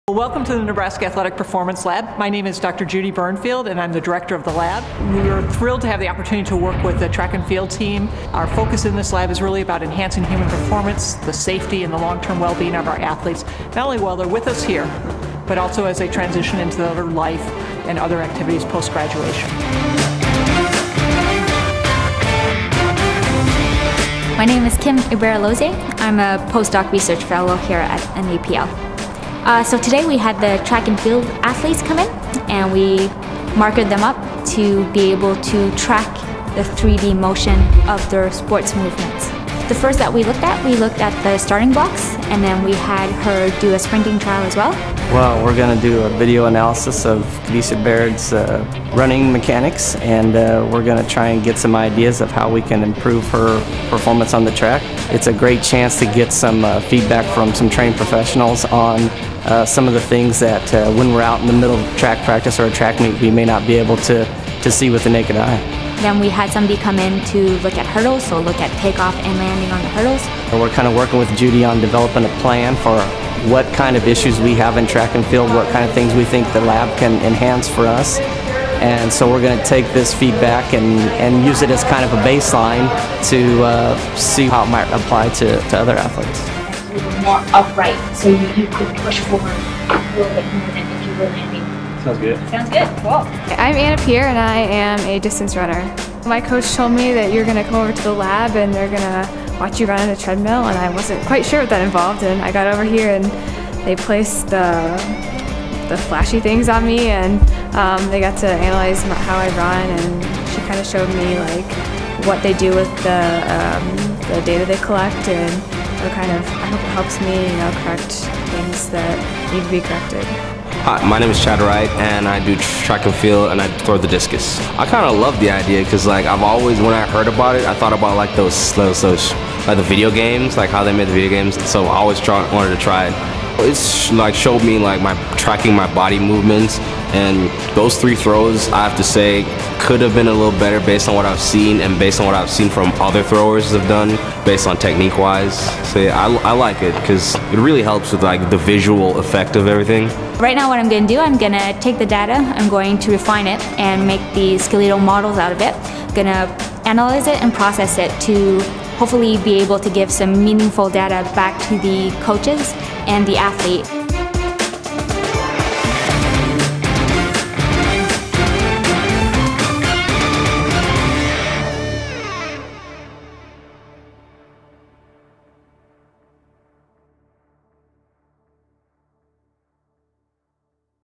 Nebraska Athletic Performance Lab - 3D Motion Capture Analysis
The Nebraska Track and Field team spends some time in the Nebraska Athletic Performance Lab conducting 3D motion capture analysis.